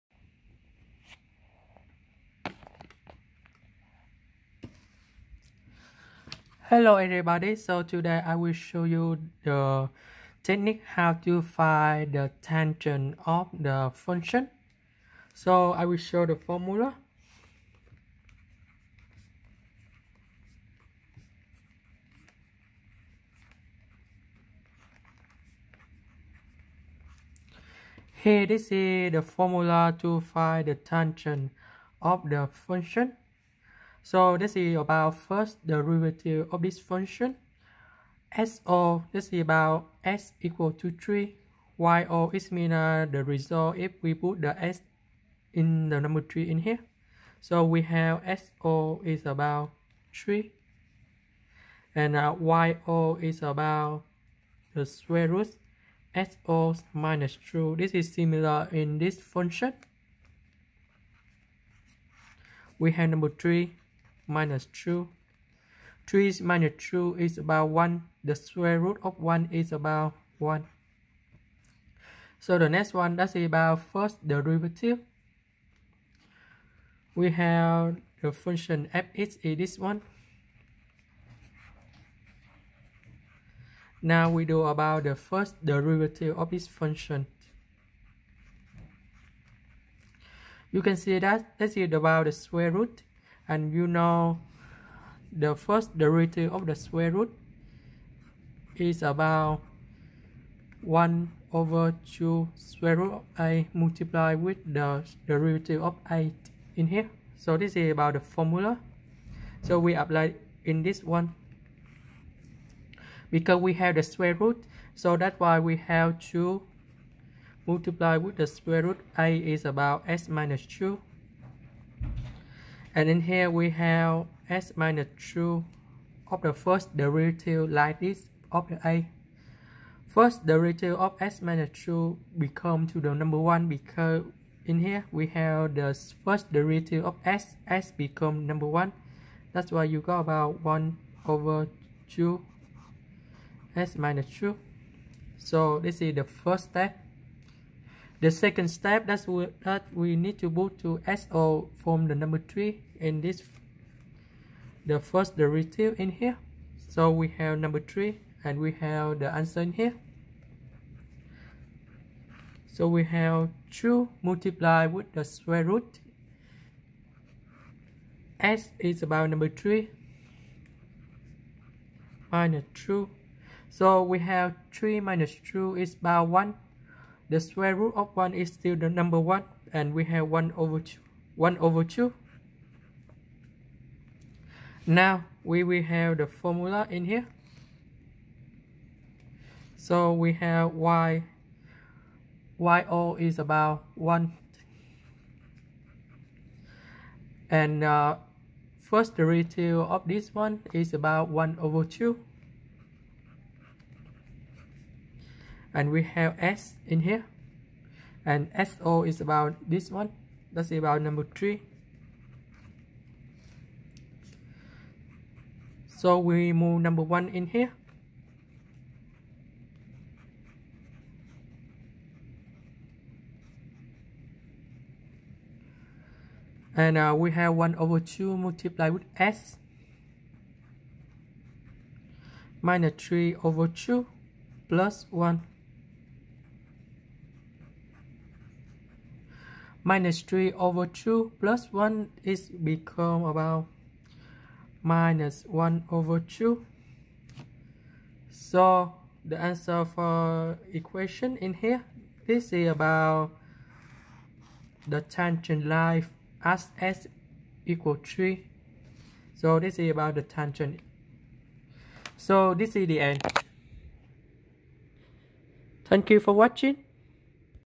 Calculus Help: Find the tangent line by first derivative.
Here is my step-by-step to find the tangent line of function at x.